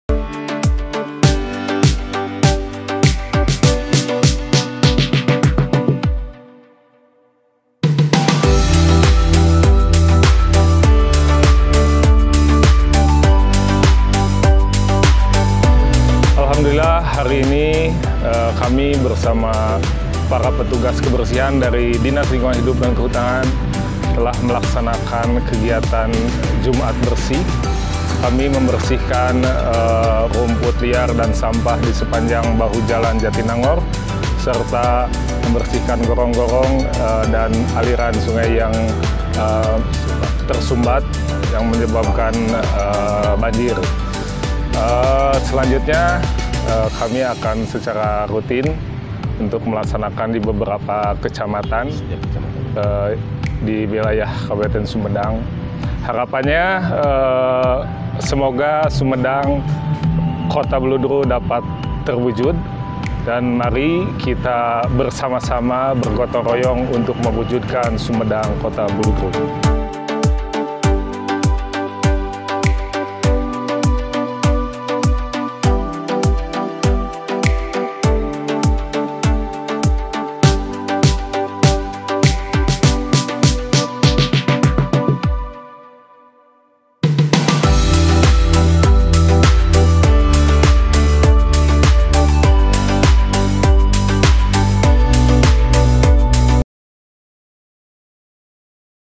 Jatinangor Bersih Menuju Sumedang Kota Buludru
Menjaga kebersihan lingkungan harus terus dilakukan. Mari bersama-sama mewujudkan kembali SUMEDANG KOTA BULUDRU.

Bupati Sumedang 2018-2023